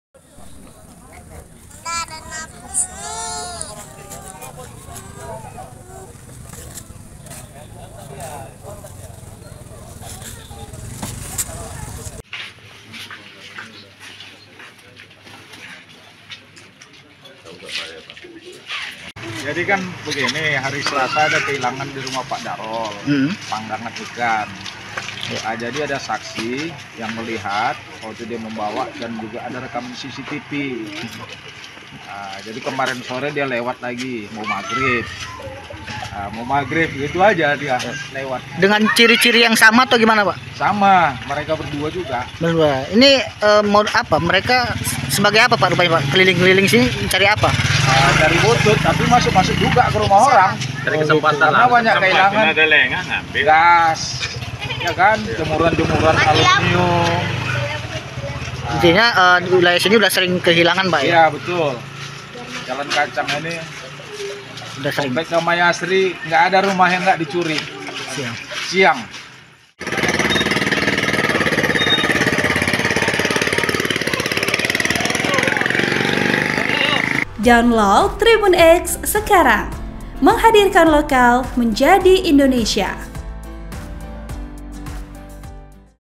Dua Pencari Barang Bekas Diamankan, Terekam CCTV Masuki Rumah Warga dan Curi Barang
TRIBUN-MEDAN.com, KISARAN - Warga Jalan Kacang, Siumbut-umbut, Kabupaten Asahan Dihebohkan dengan ditangkapnya dua orang maling yang berprofesi sebagai pencari barang bekas (botot).

Keduanya diamankan karena kedapatan warga sedang dan terekam CCTV memasuki rumah salah seorang warga.

Sehingga, saat keduanya melintas kembali, langsung diamankan oleh warga dan dibawa ke pos kemanan lingkungan.

Dalam video amatir yang diterima tribun-medan.com, keduanya dikenakan hukuman sikap salah dengan kepala di bawah. Hal ini dilakukan untuk memberikan efek merah terhadap kedua pelaku.

Robert, salah seorang warga menjelaskan, keduanya diamankan karena dihari sebelumnya, seorang warga kehilangan barang dan melihat rekaman cctv ternyata pelakunya adalah dua orang pria pencari barang bekas.

"Kemudian, dua orang pelaku yang terekam cctv melintas dan langsung ditangkap warga. Warga yang tersulut emosi sempat hendak menganiaya keduanya, namun berhasil dihalangi," kata Robert, Jumat (1/12/2023) malam.